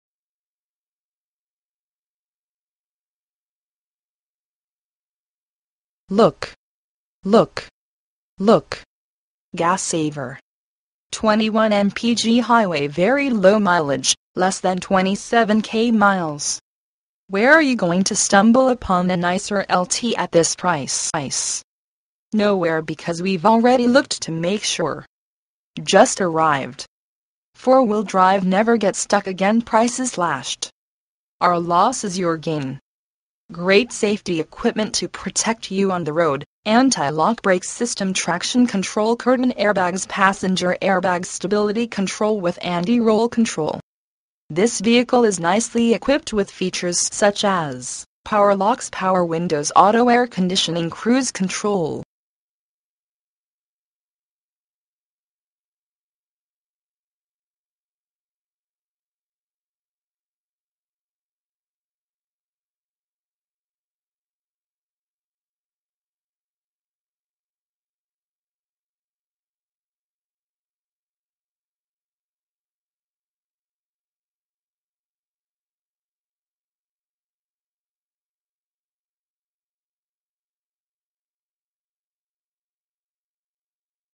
2011 Chevrolet Silverado 1500 - Bob Allen Motor Mall - Danville, KY 40422
Bob Allen Motor Mall
725 Maple Ave

For Sale: 2011 Chevrolet Silverado 1500
VIN: 1GCPKSE31BF230718
Engine: 5.3L V8
Drivetrain:
Transmission: Automatic 6-Speed
Mileage: 26,410
Color: Black (exterior)  (interior)

Features:
Look!! Look!! Look!! Gassss saverrrr!!! 21 MPG Hwy! Very Low Mileage: LESS THAN 27k miles!!! Where are you going to stumble upon a nicer LT at this price? Nowhere because we've already looked to make sure.. Just Arrived!! 4 Wheel Drive never get stuck again*** PRICES SLASHED!!! OUR LOSS IS YOUR GAIN!!! Great safety equipment to protect you on the road: Anti-lock Brake System Traction control Curtain airbags Passenger Airbag Stability control - With anti-roll control...This Vehicle is nicely equipped with features such as: Power locks Power windows Auto Air conditioning Cruise control...